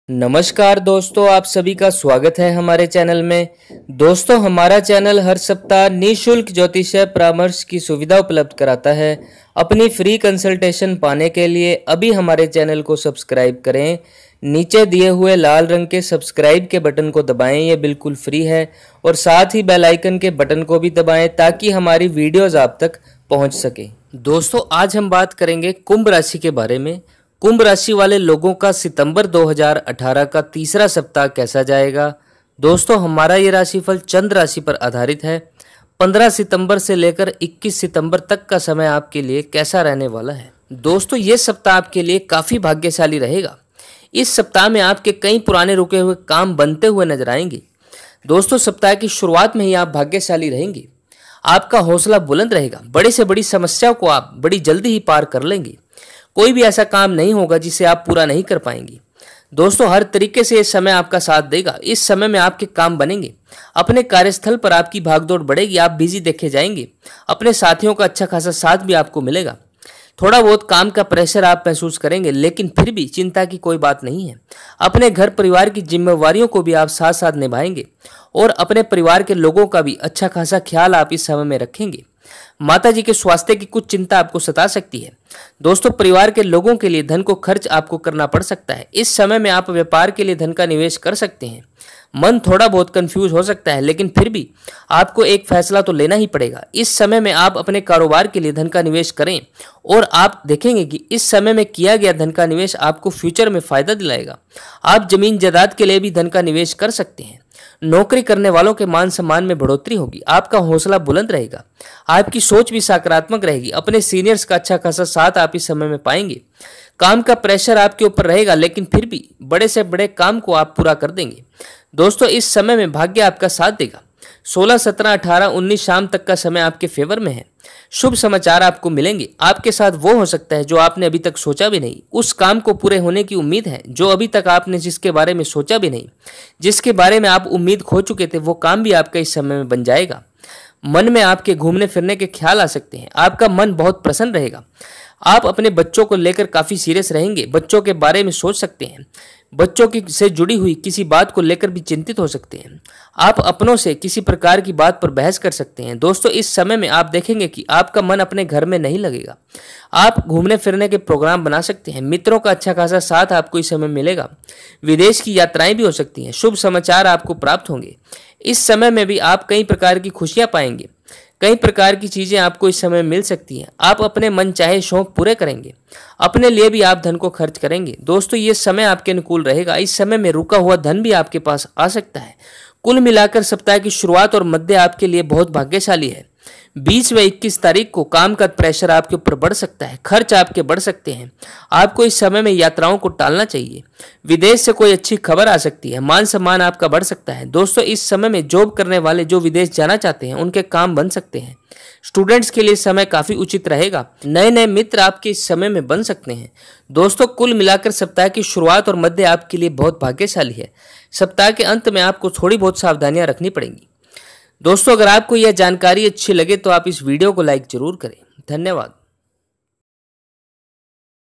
कुंभ राशि तीसरा सप्ताह सितम्बर 2018/Kumbh rashifal 15 september se 21 september
Kumbh rashi tisra saptah september 2018
Kumbh saptahik rashifal
Saptahik rashifal kumbh